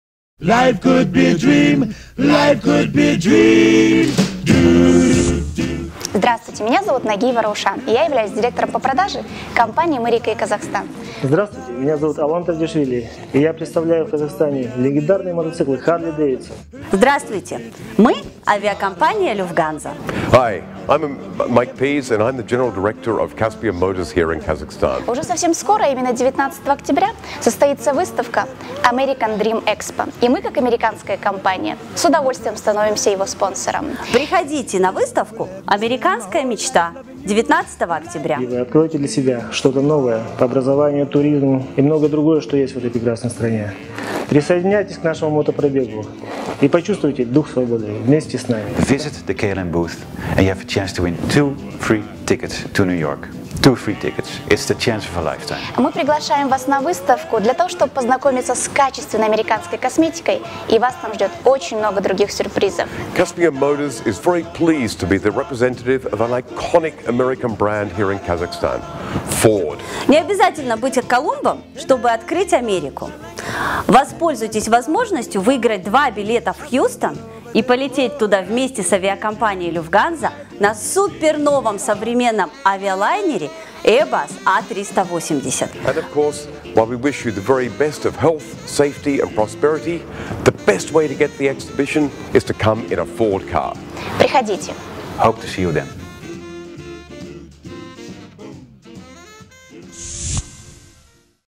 Американская мечта, или почему стоит посетить American dream Expo
19 октября 2013г. в «Алматы Тауэрс» состоится грандиозная туристическая и образовательная выставка American Dream Expo («Американская мечта»). Представители более 30 американских университетов расскажут о своих образовательных программах, стипендиях и о процессе подачи документов. У вас есть возможность узнать информацию из первых рук и получить бесплатную академическую консультацию EducationUSA, задать вопросы о получении визы в США и встретиться с выпускниками американских университетов в Алматы.
Туристические агентства предложат отпускные пакеты и горячие путевки в популярные города Америки.
Посетителей ждет розыгрыш 8 авиа-билетов в обе стороны в самые популярные города Америки от основных авиаперевозчиков в Казахстане и другие сюрпризы.
Мы обеспечим Американское настроение в течение всего мероприятия!